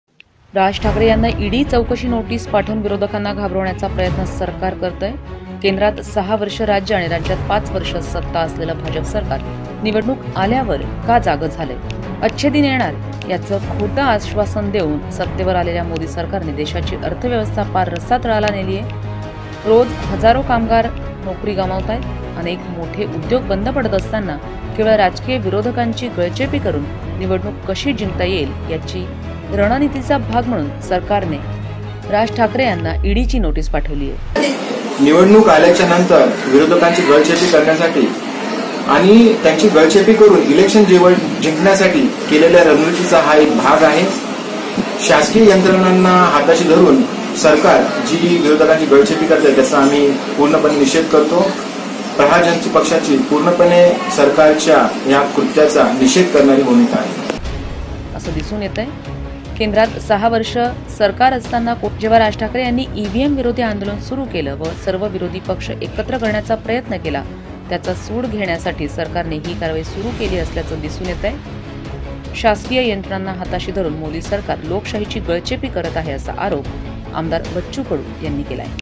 शासकीय यंत्रणांना हाताशी धरून मोदी सरकार लोकशाहीची गळचेपी - आमदार बच्चू कडू
राज ठाकरे याना ईडी चौकशी नोटीस पाठवून विरोधकांना घाबरवण्याचा प्रयत्न
केंद्रात 6 वर्ष व राज्यात 5 वर्ष सत्ता असलेले भाजप सरकार निवडणूक आल्यावर का जागे झाले?
अच्छे दिन येणार याचे खोटे आश्वासन देऊन सत्तेवर आल्यावर मोदी सरकारने देशाची अर्थव्यवस्था पार रसातळाला नेली